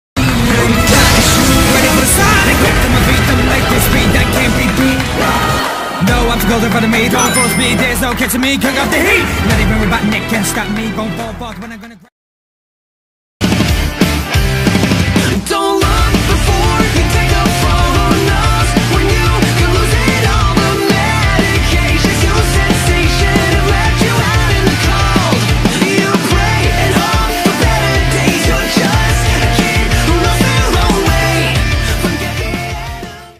YouTube intro and outro for 2020
This will be the new intro and outro for 2020 I hope you like it.
The thumbnail for this video will also be the new profile picture for 2020 but without the text